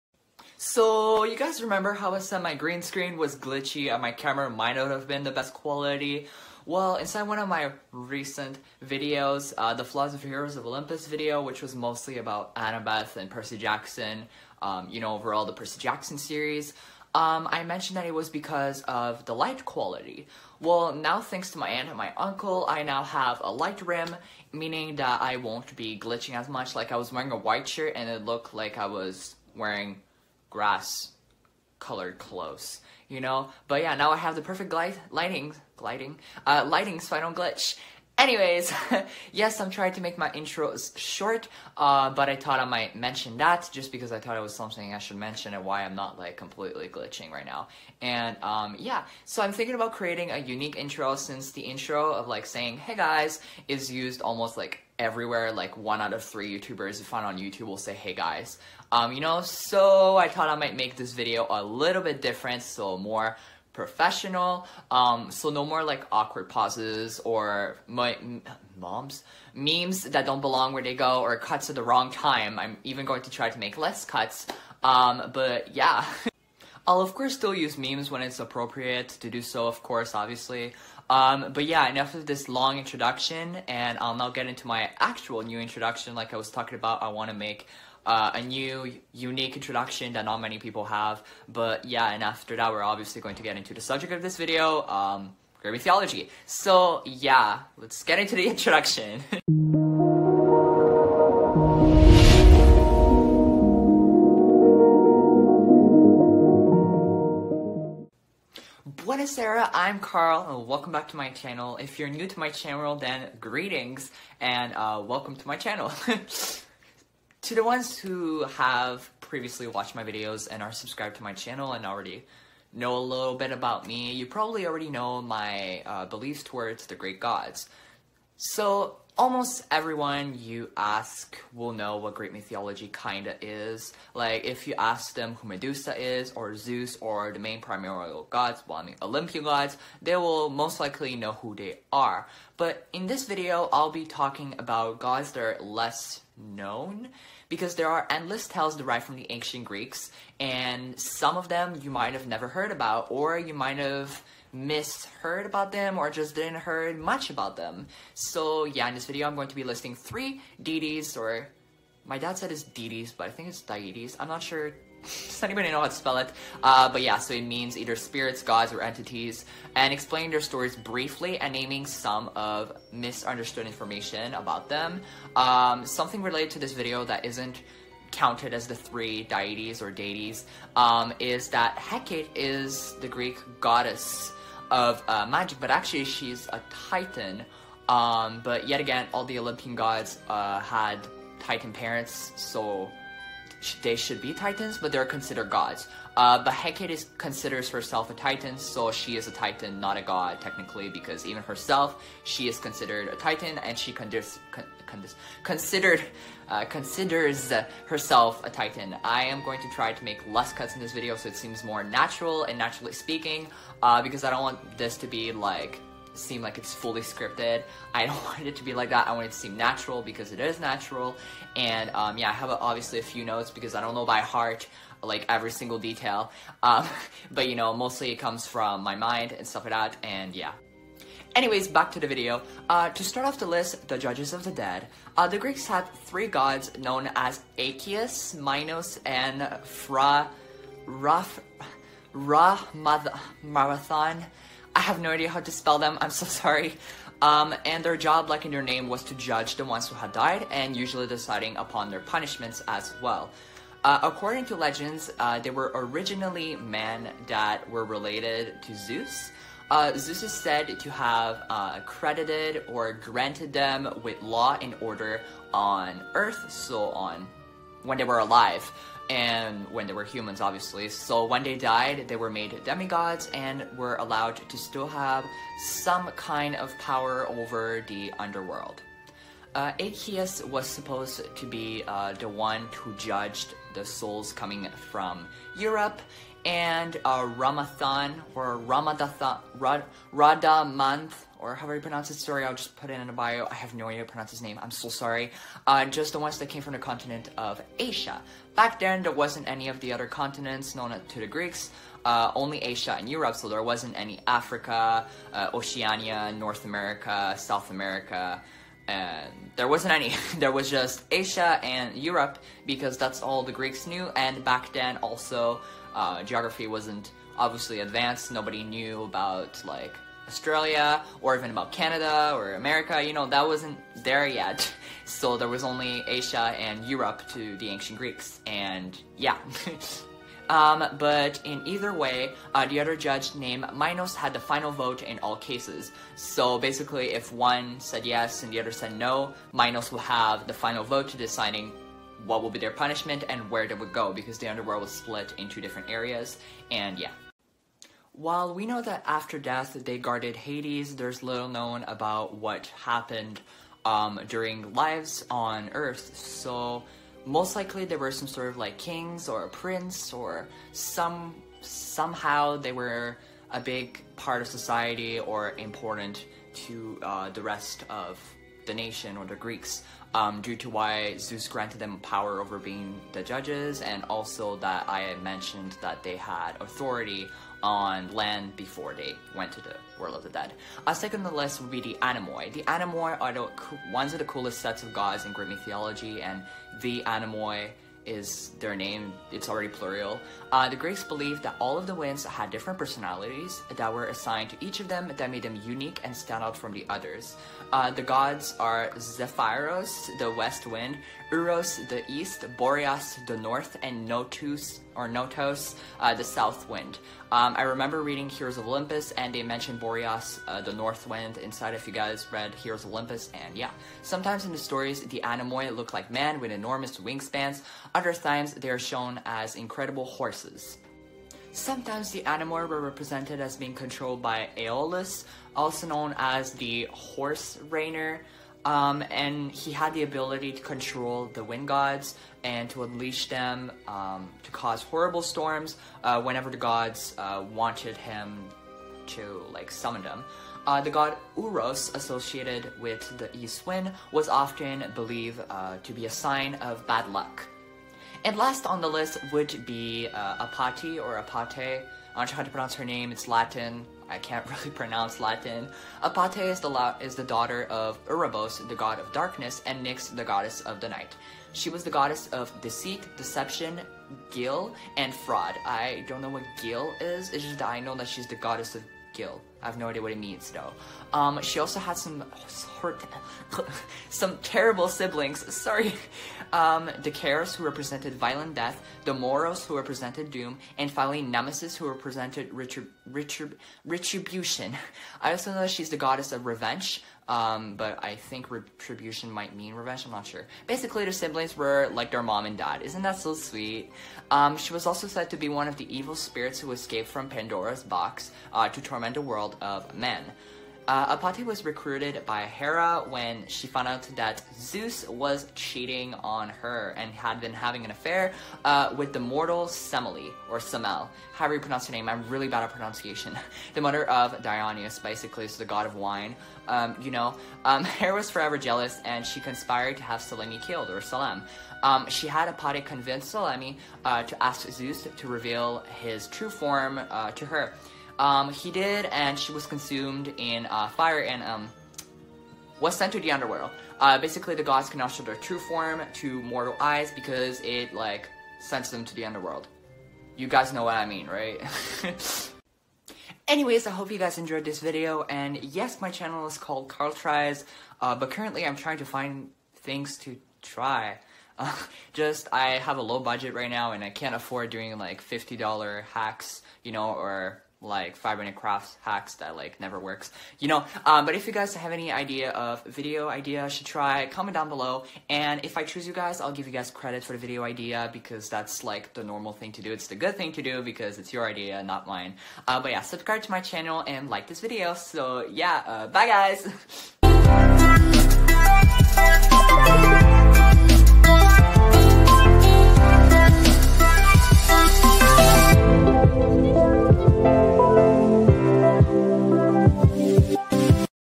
Meet the Relatively Unknown Gods | Greek mythology
Buona sera!👻💙 In this video I will be talking about three less known or less talked about deities from Greek mythology!👻💙 Including the wind gods, the judges of the dead and Apate! Like the video and subscribe to my channel!👻💙(Also, I just did a bit more research, and the ancient Greeks had African slaves, meaning, that they most likely knew about Africa, but they still did not have a judge to judge souls coming from Africa, which is why I’m still wondering if they actually knew about Africa.)